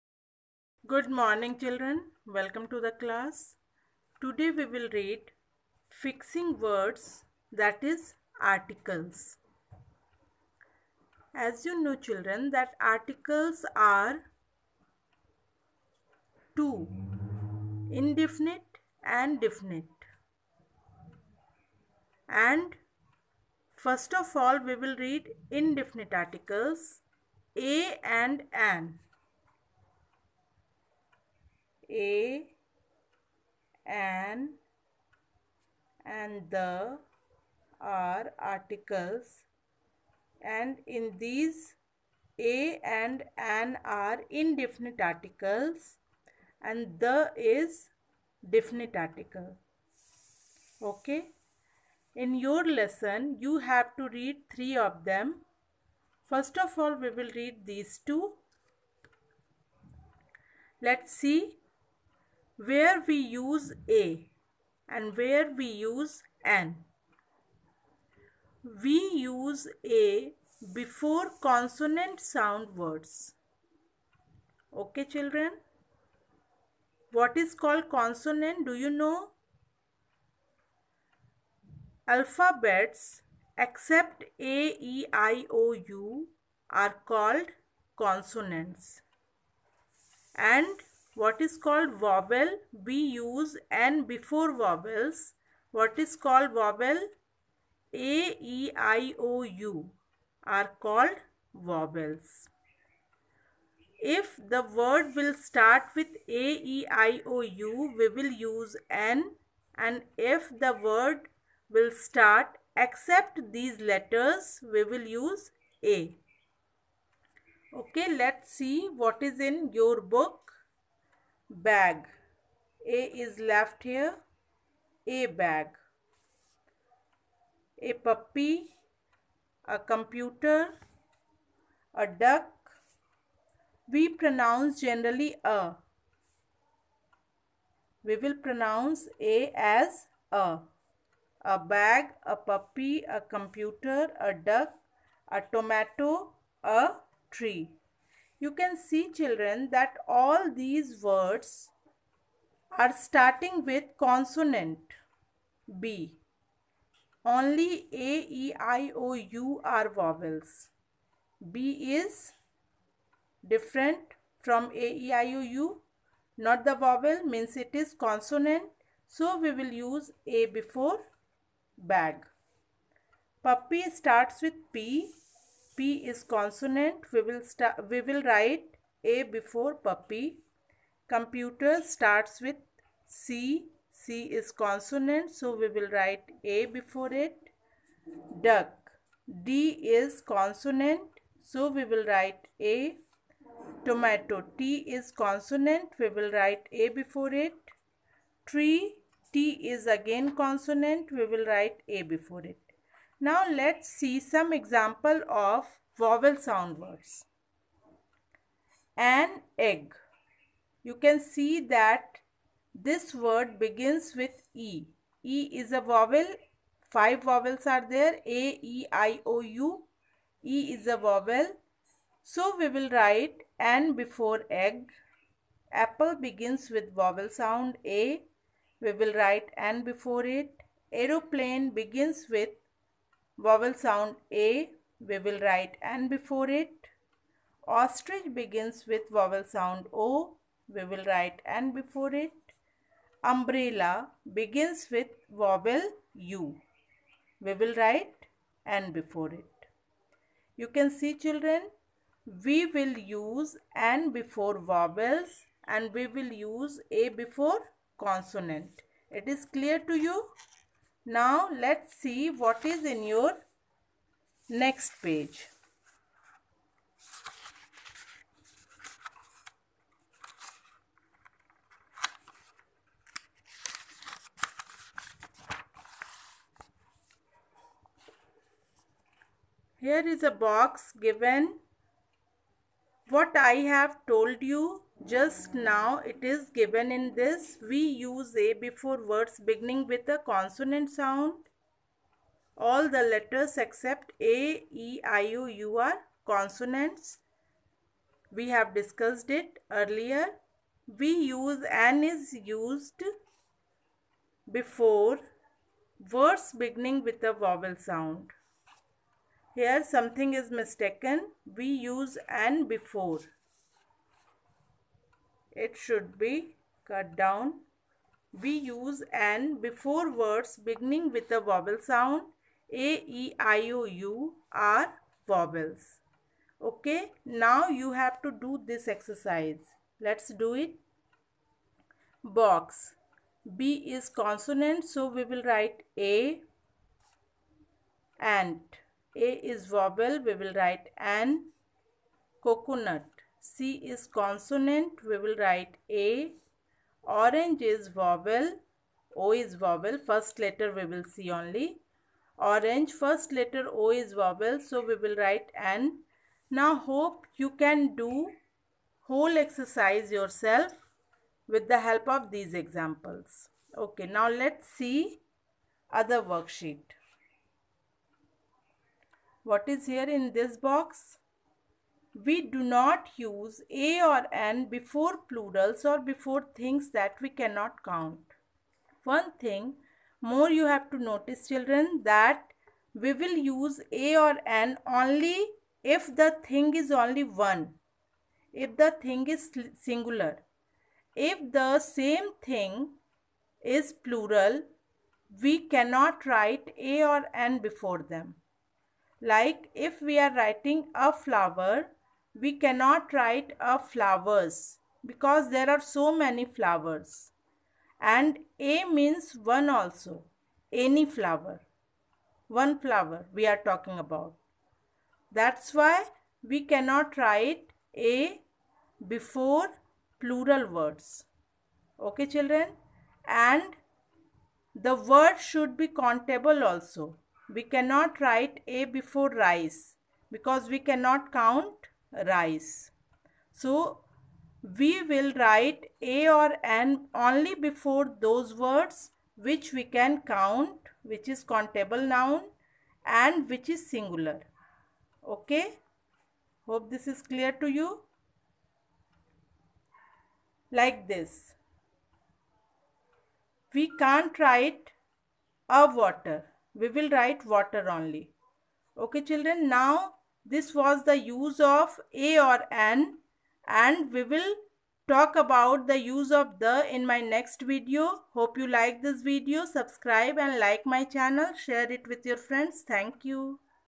Fixing words(Articles) || Uses of 'a' and 'an' || Class 3 || Vandana Ma'am
Fixing words(Articles) .(part 1)

This  video is about indefinite articles 'a' and 'an' with their uses  explained by Vandana Jha

The topic is easy, just practice more